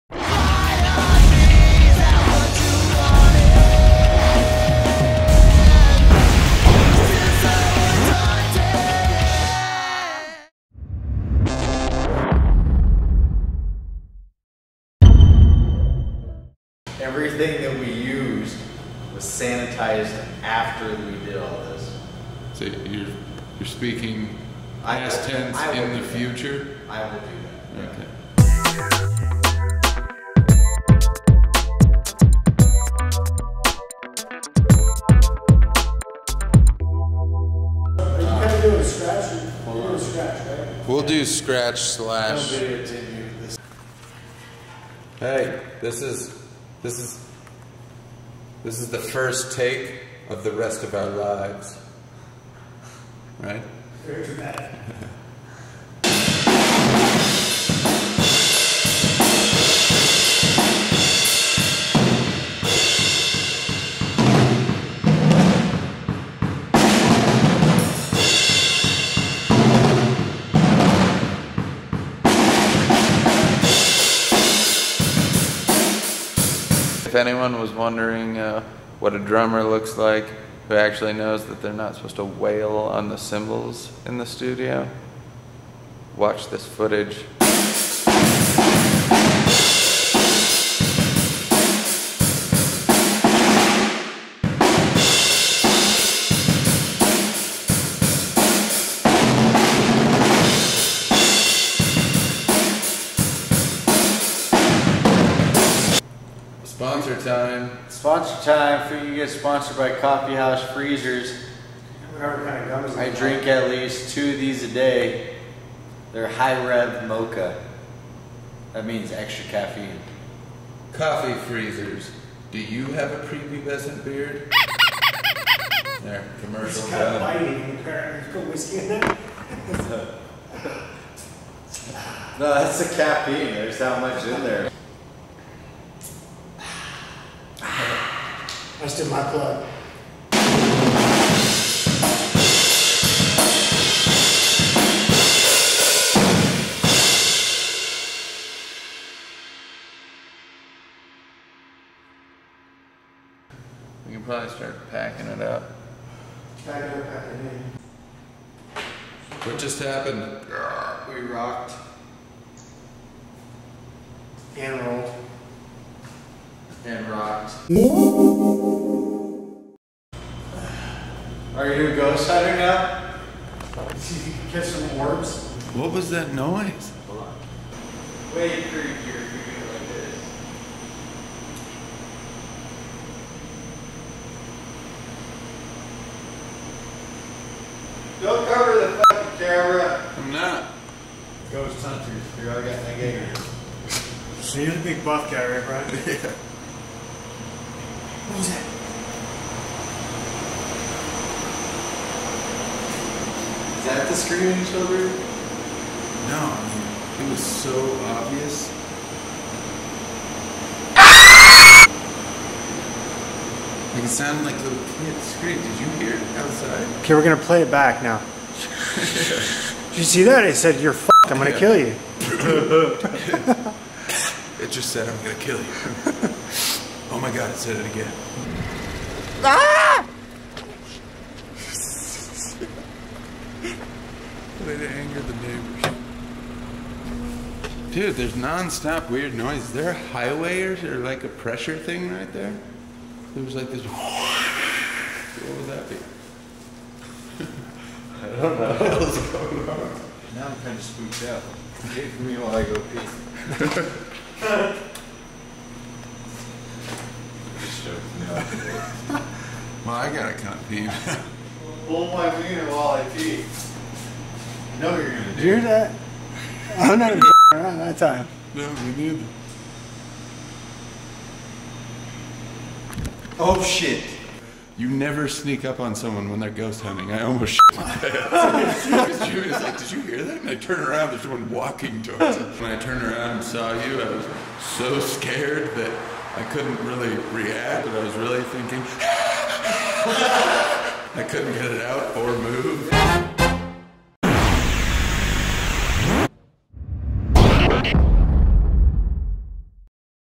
Drums & Ghosts: Episode 1 of Heart & Soul Radio - Making The Album
Join Heart & Soul Radio as they record their new album.  In this episode, Jimmy tracks drums in a meat processing plant, and the guys hear terrifying ghost screams of children and get a surprise.